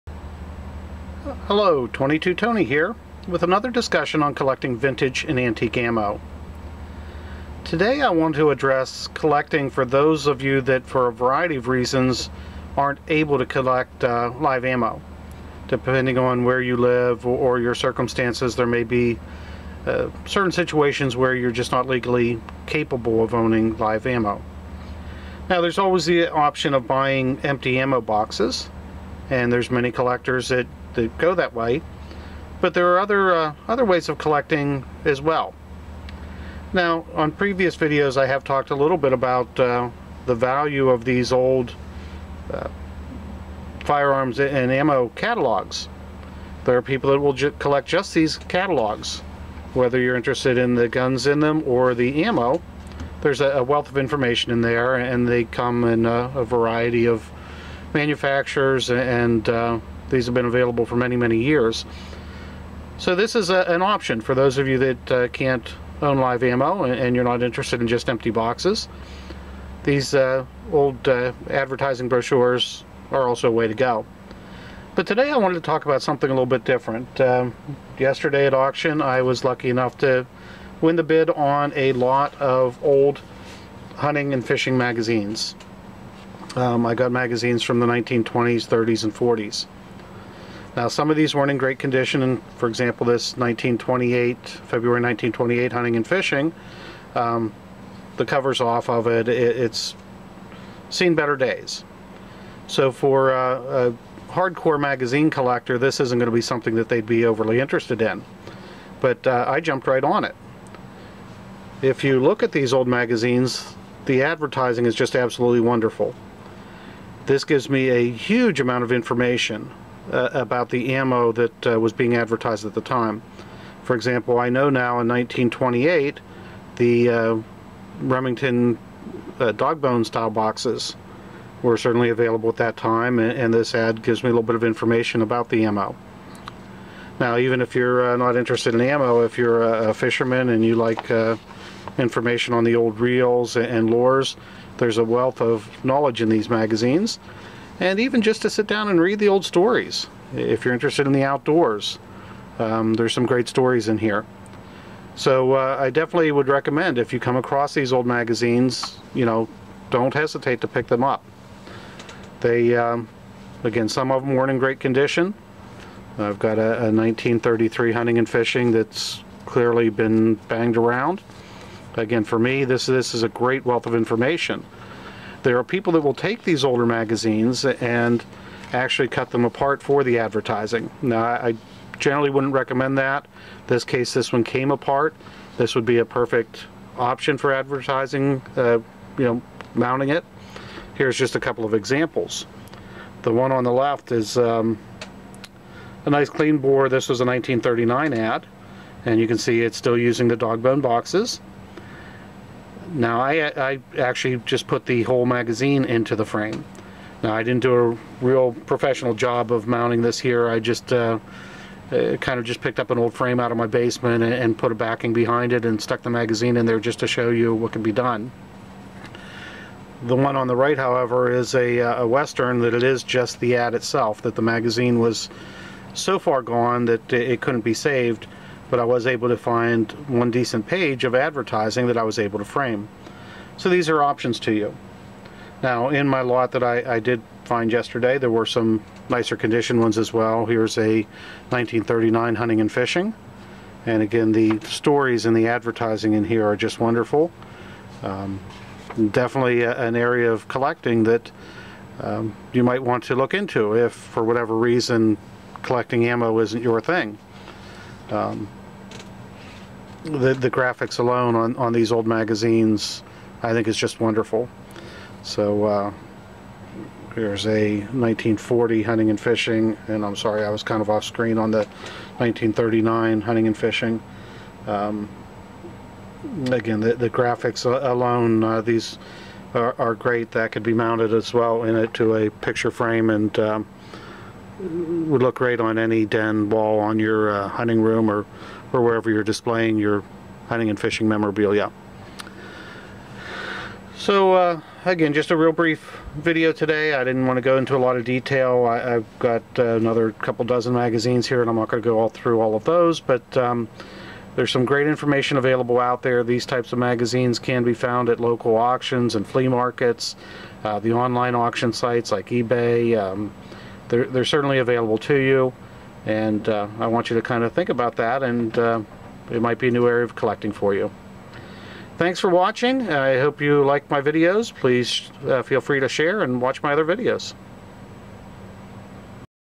Collecting vintage Hunting and Fishing Magazines. Ammo Boxes
Another way of collecting vintage and antique ammo.